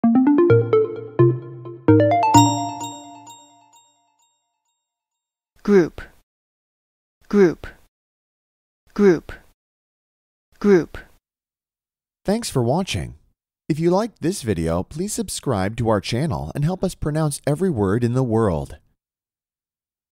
How To Pronounce Group - Pronunciation Academy
Learn how to pronounce Group

This is the *English* pronunciation of the word Group.